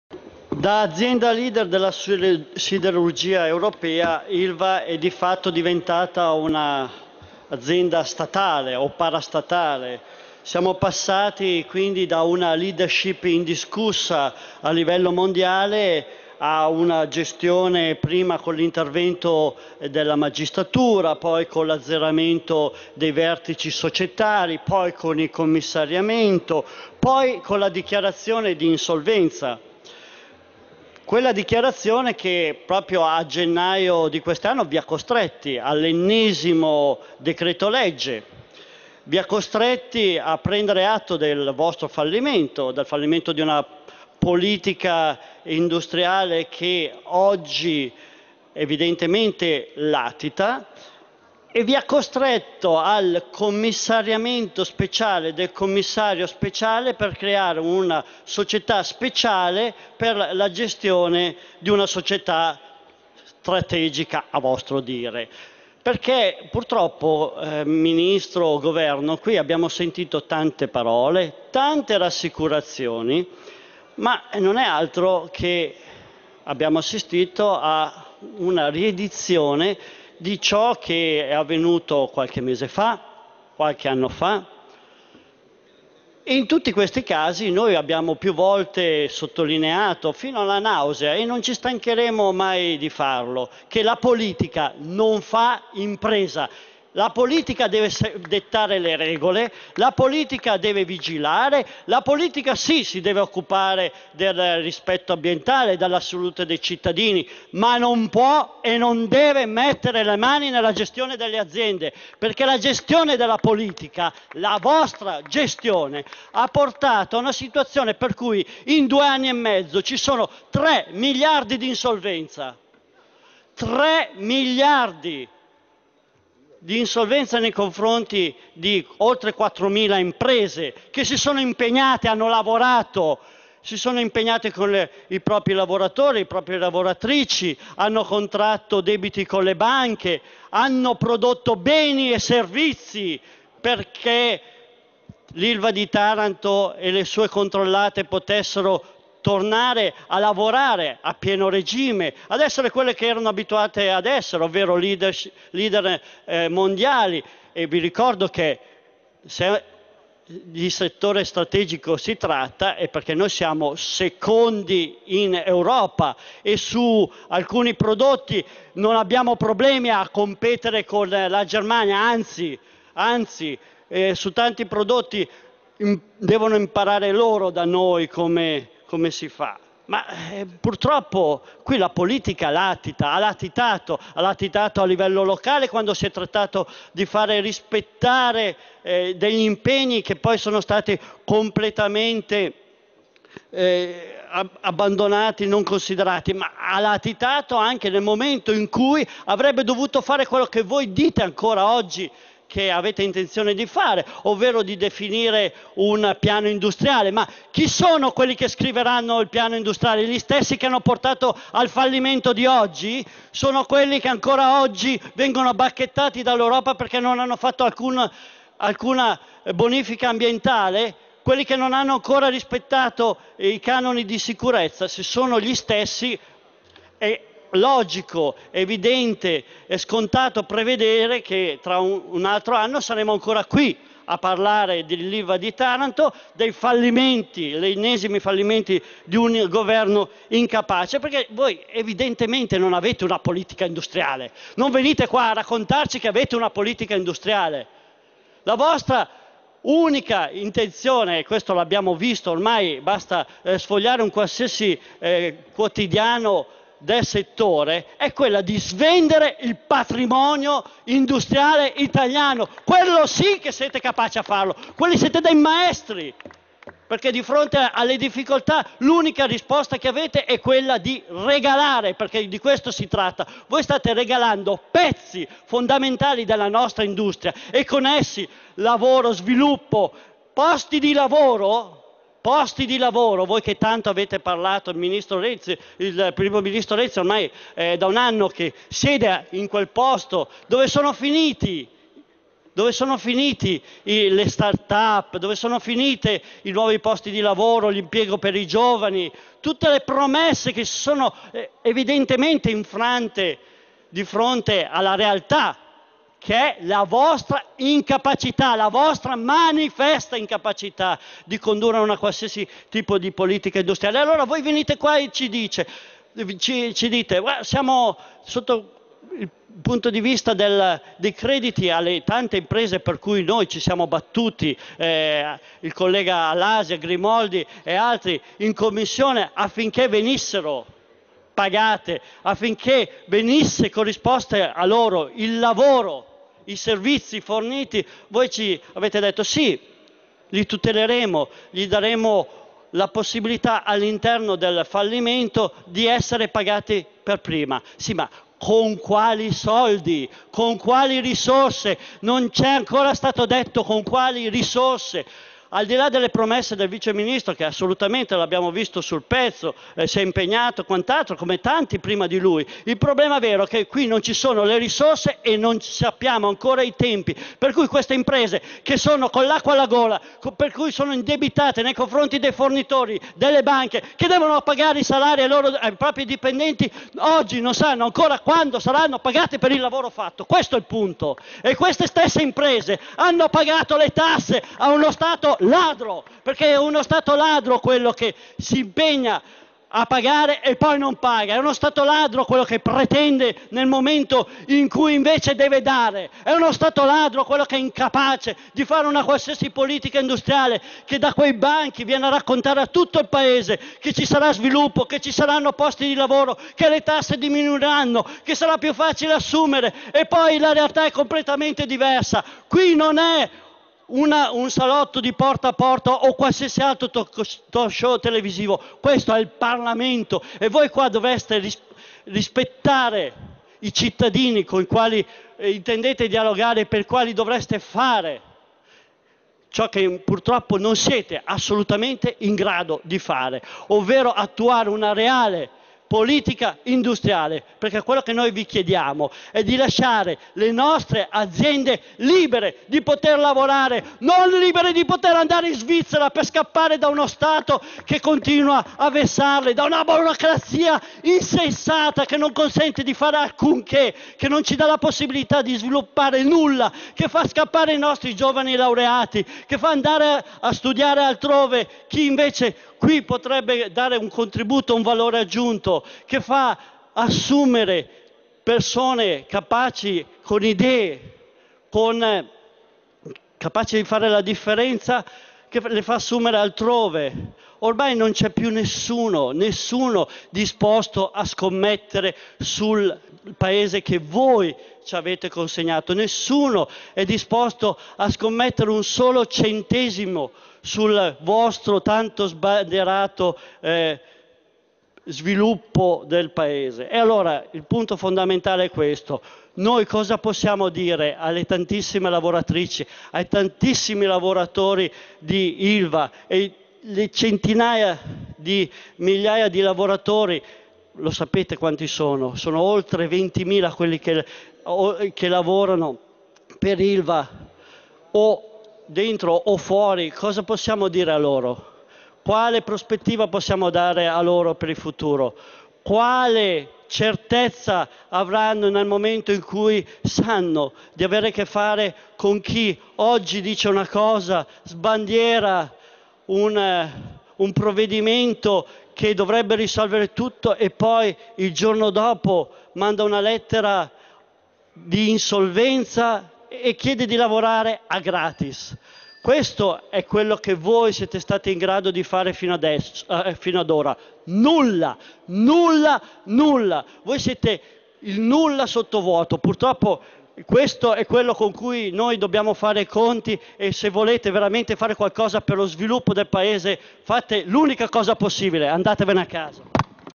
Ilva - Caparini: il governo Renzi è il nulla sottovuoto
ROMA, 3 MAR - “Quattromila aziende dell’indotto dell’Ilva attendono da anni i pagamento dello Stato ladro, che ha preteso le tasse e non ha pagato i fornitori. L’ingerenza della politica nella gestione dell’Ilva sta desertificando il patrimonio siderurgico del paese, un tempo ai vertici mondiali. È l’ennesimo fallimento di un governo che merita solo di andare a casa”. Così il deputato leghista Davide Caparini nel corso delle dichiarazioni di voto (contrario) al decretoIlva. Caparini ha accusato gli ultimi esecutivi di sinistra di “aver ridotto sul lastrico l’azienda, producendo 3 miliardi di insolvenze in due anni e mezzo”, e “le 4mila imprese fornitrici – con sede per lo più al Nord – esposte con banche e dipendenti, gravate dalle tasse su incassi mai ottenuti, e in eterna attesa dei pagamenti dello Stato”.
“Non basta aver inserito i crediti nel calderone della gestione
fallimentare, il governo non ha previsto le risorse”, ha avvertito
Caparini. Per il parlamentare del Carroccio il governo, sul caso Ilva,
“ha dimostrato di non avere una politica industriale” e di “voler
azzerare il patrimonio industriale del paese”. “Le nostre aziende
chiedono solo di poter lavorare, i nostri laureati chiedono di non
dover scappare all’estero per avere prospettive”. “Il governo sta
impedendo alle nostre imprese di lavorare e di creare lavoro, se vuole far qualcosa di utile per il paese ha una sola strada: andare a casa”.